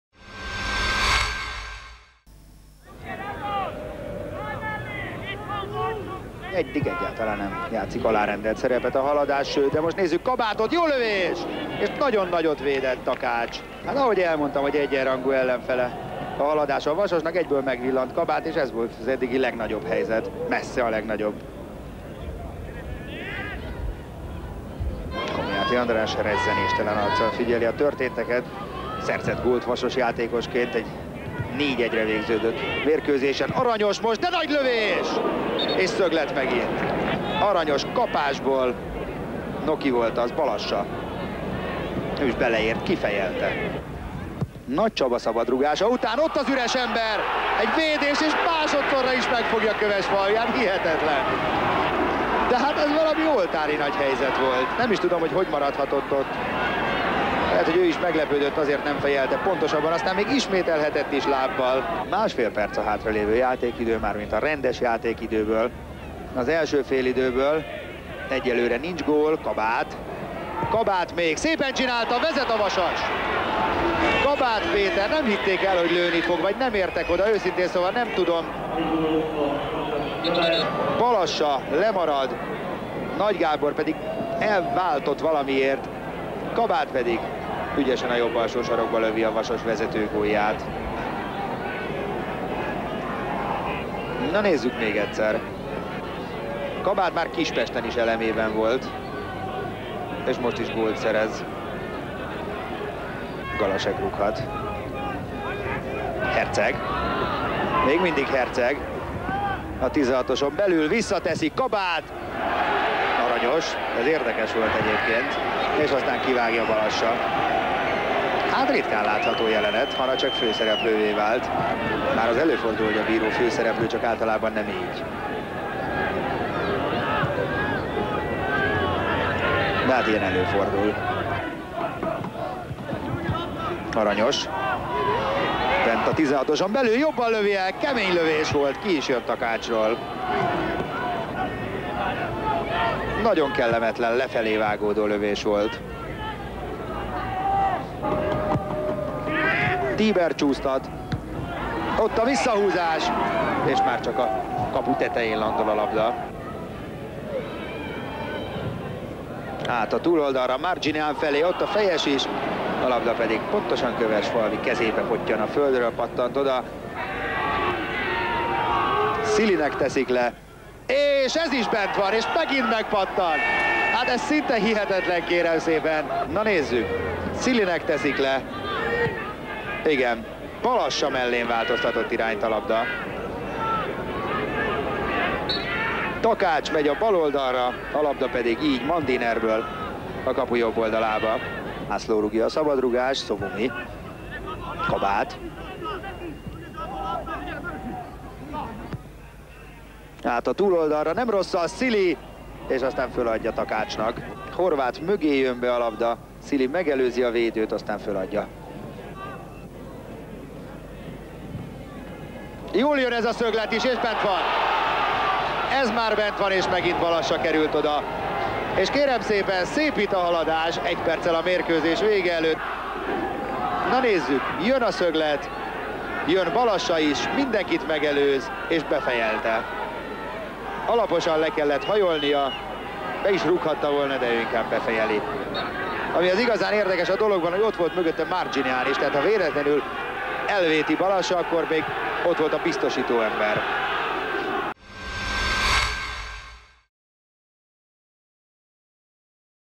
Haladás-Vasas | 1-2 | 2000. 03. 10 | MLSZ TV Archív
"1999/2000, NB I, 21. forduló. A Vasas a harmadik helyen állt, a Haladás a 13. volt.